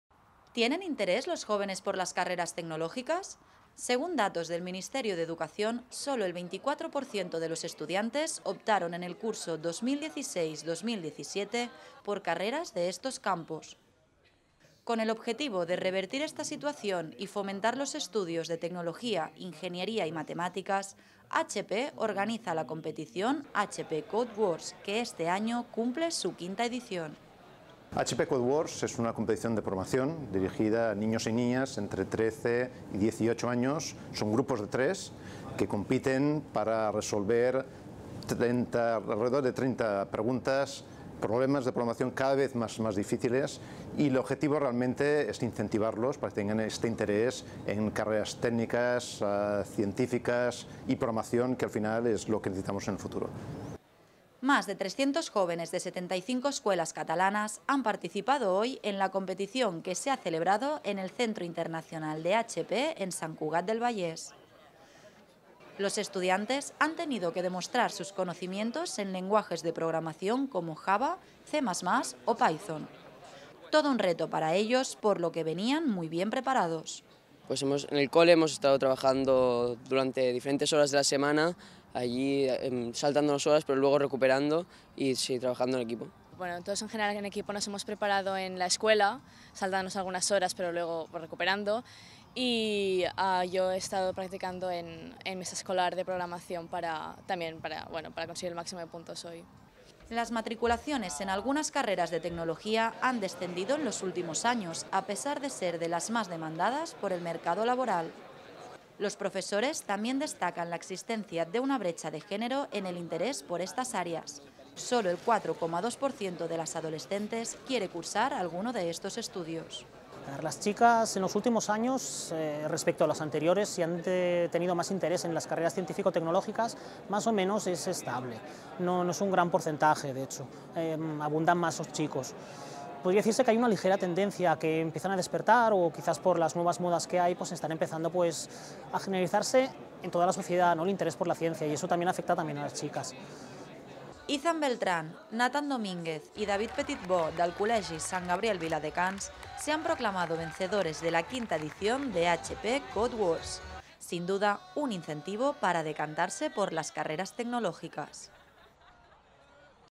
Más de 300 jóvenes participan en el HP CodeWars de programación en su quinta edición
¿Tienen interés los jóvenes por las carreras tecnológicas? Según datos del Ministerio de Educación, sólo el 24 % de los estudiantes optaron en el curso 2016-17 por carreras de estos campos. Con el objetivo de revertir esta situación y fomentar los estudios de Tecnología, ingeniería y matemáticas, HP organiza la competición HP Code Wars, que este año cumple su quinta edición.

Más de 300 jóvenes de 75 escuelas catalanas han participado hoy en la competición que se ha celebrado en el Centro Internacional de HP en Sant Cugat del Vallés. Los estudiantes han tenido que demostrar sus conocimientos en lenguajes de programación como Java, C++ o Python. Todo un reto para ellos, por lo que venían muy bien preparados.

Las matriculaciones en algunas carreras de tecnología han descendido en los últimos años, a pesar de ser de las más demandadas por el mercado laboral. Los profesores también destacan la existencia de una brecha de género en el interés por estas áreas. Sólo el 4,2% de las adolescentes quiere cursar alguno de estos estudios.

Izan Beltran, Natan Domínguez y David Petitbò, del Col•legi Sant Gabriel Viladecans, se han proclamado vencedores de la quinta edición de HP CodeWars. Sin duda, un incentivo para decantarse por las carreras tecnológicas.